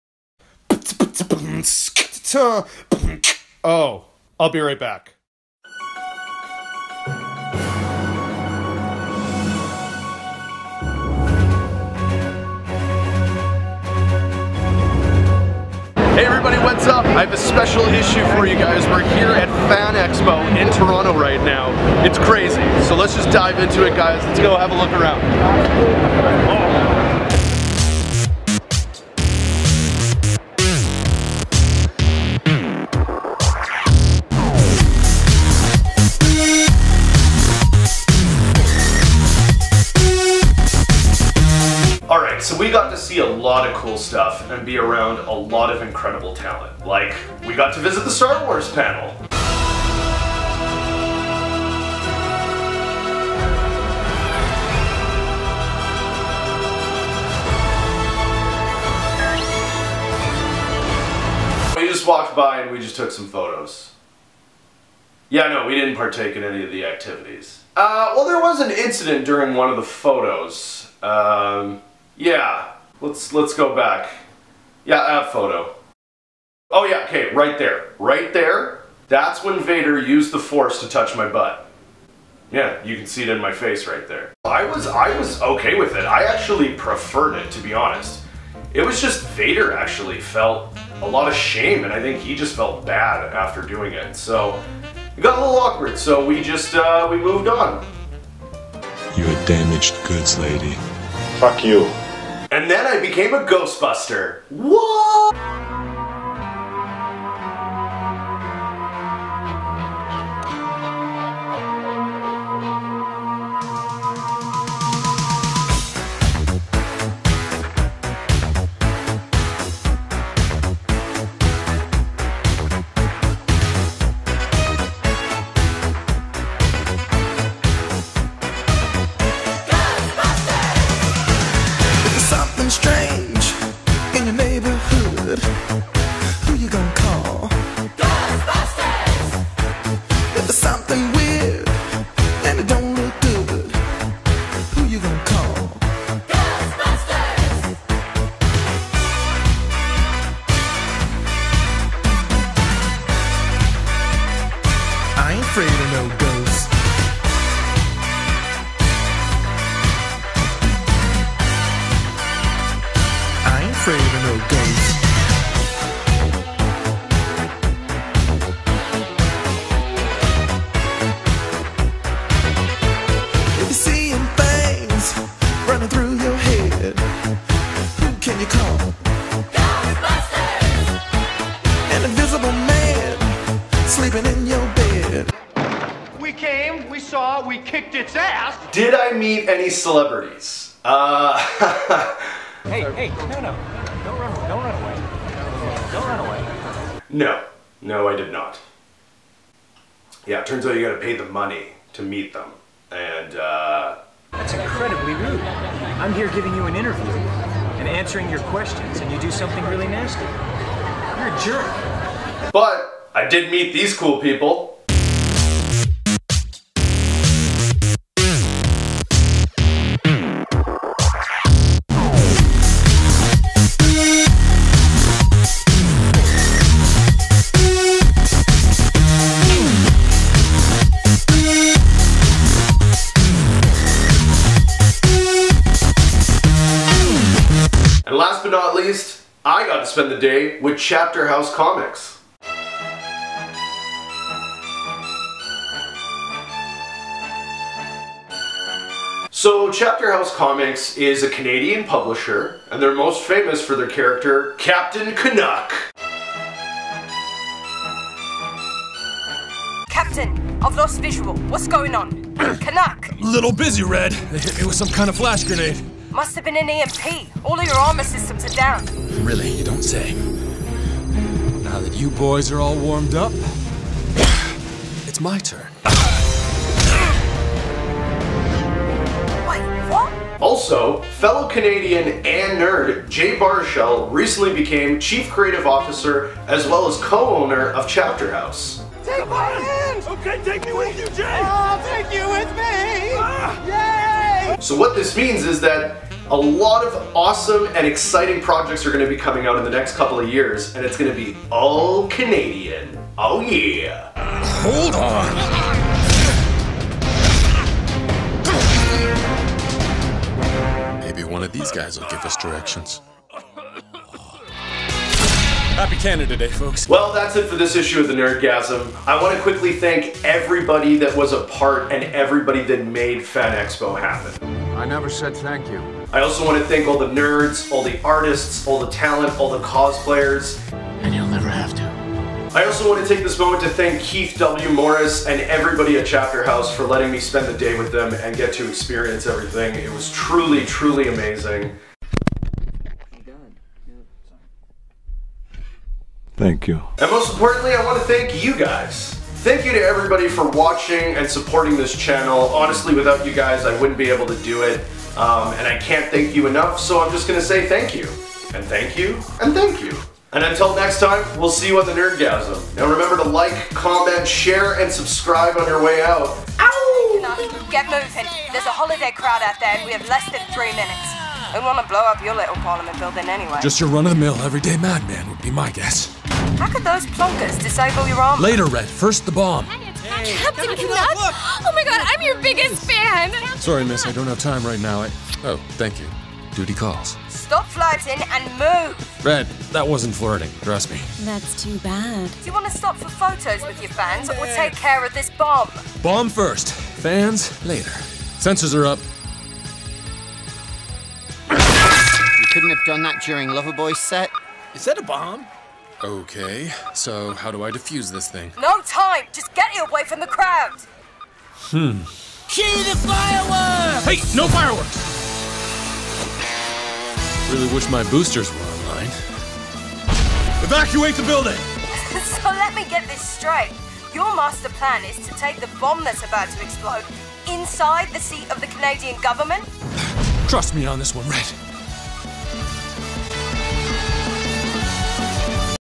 NG: Issue #17 - The FANEXPO CANADA Episode! (2017)
On this Issue of The Nerdgasm I got to make a surprise visit to FanExpo Canada in Toronto! I became a Ghostbuster, had an incident with Darth Vader and I spent the day with Chapterhouse Comics! But...

Did I meet any celebrities? Press play to find out...

YouTube.com/cfbomb